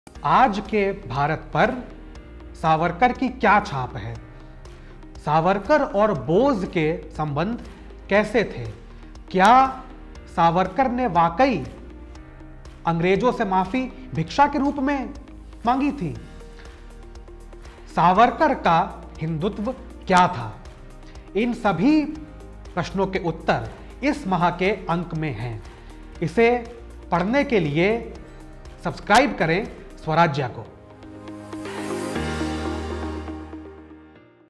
Everything Savarkar: What You'll Find In Swarajya's September Print Issue
Swarajya's September 2019 edition focuses its glance on Savarkar. Very few Indians know about him to any meaningful extent. Yet opinions on him are aplenty.

This edition will hopefully serve as a window into his life.